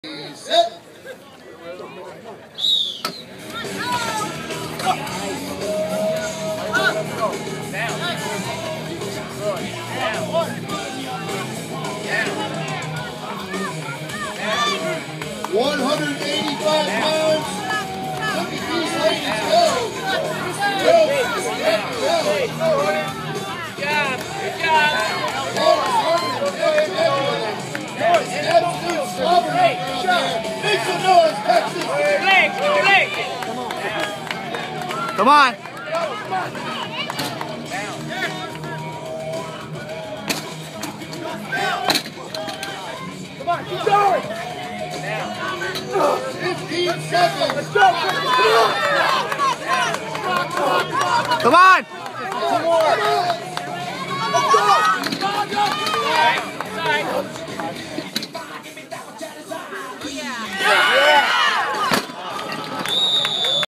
Kristin Rhodes America's Strongest Woman National's 2013 Axle Press 185 lbs
185 lb axle press for reps